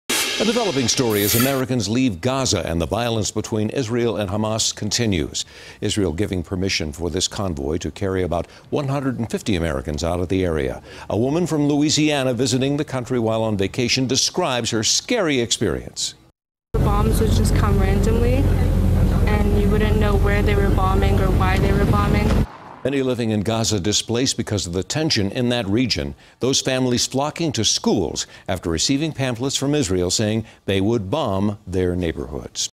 Americans flee Gaza to avoid violence
Americans living in the Gaza Strip were escorted to safety in Israel on Sunday.